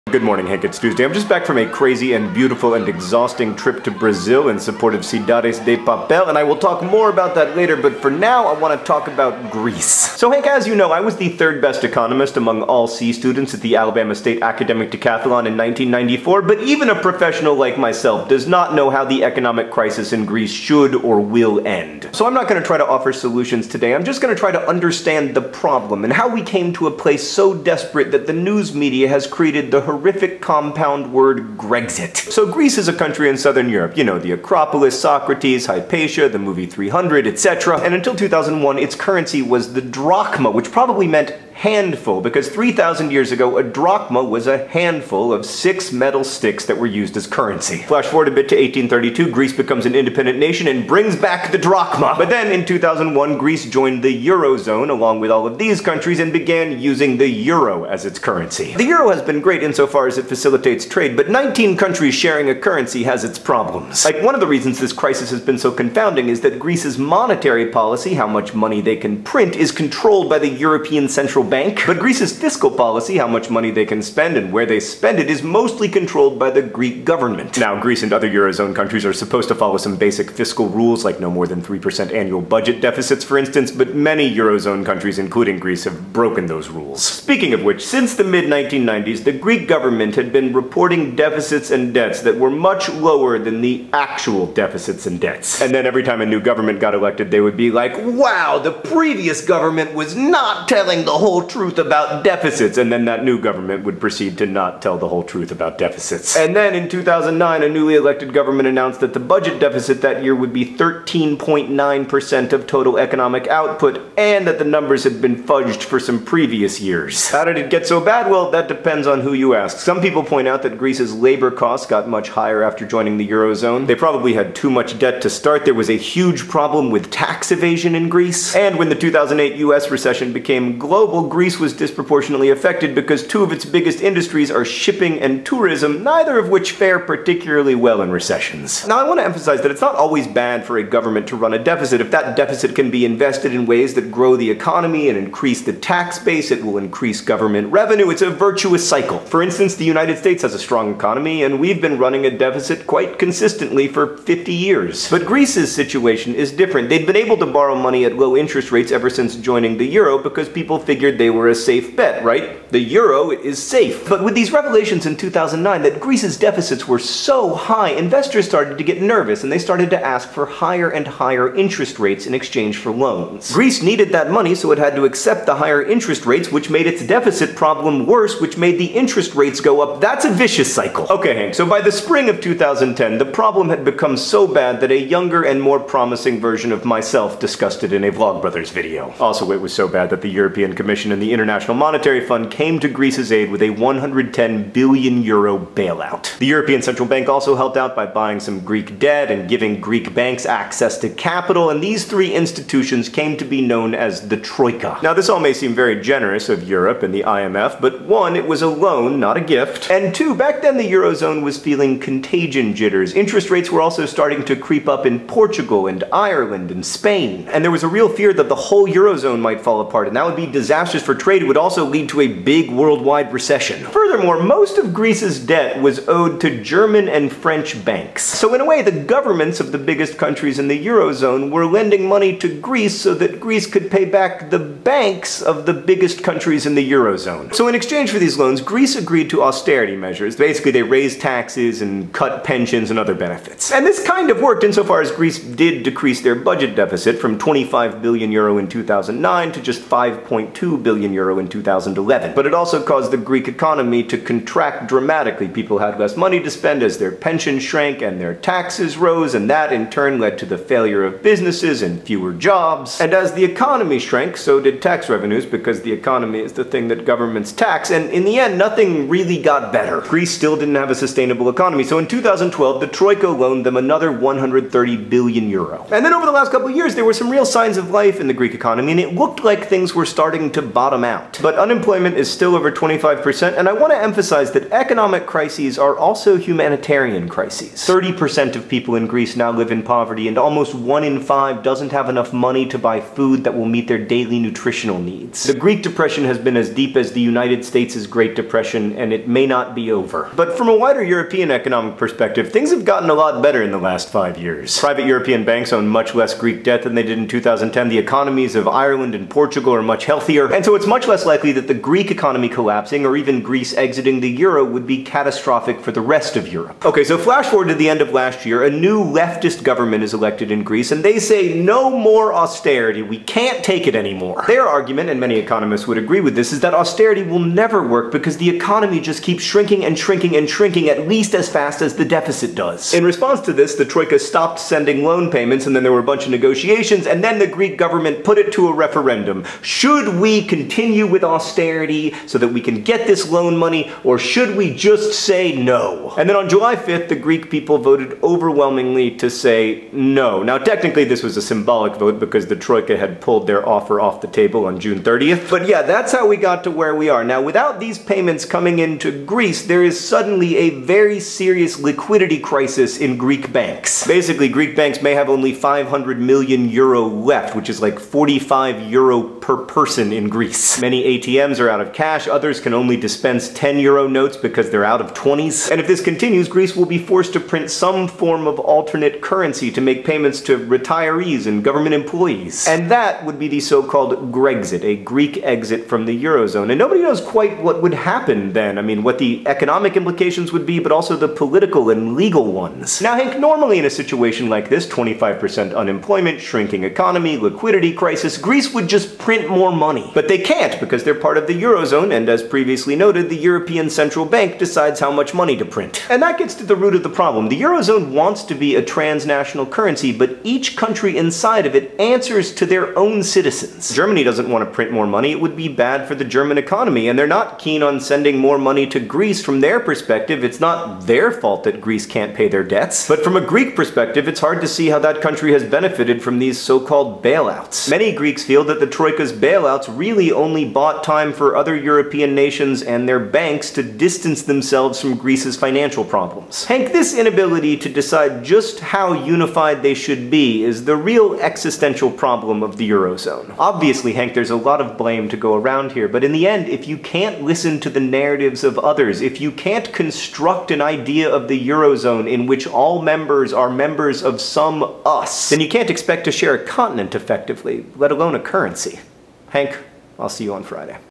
Understanding the Financial Crisis in Greece
In which John Green discusses the history of Greece's deficit and debt problems, the challenges of adopting the Euro and living with the Eurozone's monetary policy, and the possibility of the so-called Grexit--a Greek exit from the Euro.

The Economist's excellent coverage of Greece, bailouts, debt woes, and how the banking system works now and especially

And thanks very much to Rosianna for all of her help gathering facts and images. All mistakes, as always, are my own.